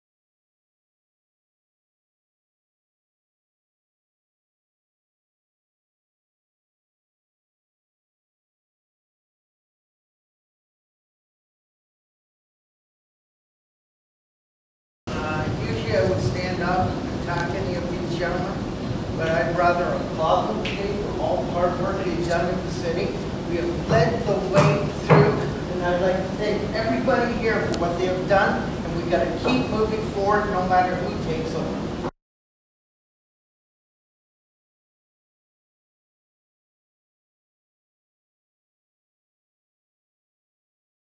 Dale Jodoin on Disabilities issues
Dale Jodoin speaks on Disabilities issues at the Diversity Debate at the UOIT.